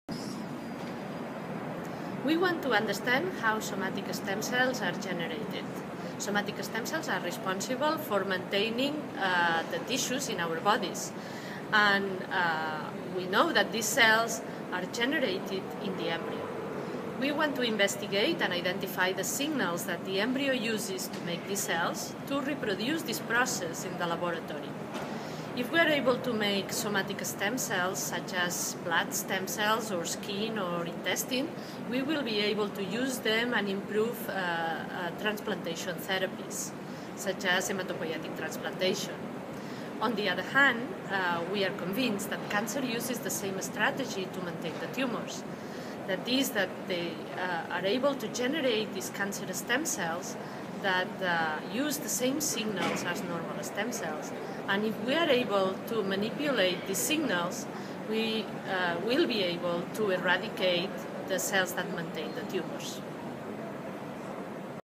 Science at PRBB: Anna Bigas' Stem cells and cancer research group (IMIM-Hospital del Mar)
A one minute summary of the work on stem cells done by Anna Bigas' group at the IMIM - Hospital del Mar Research Institute. Produced by the Barcelona Biomedical Research Park (PRBB). hhpt://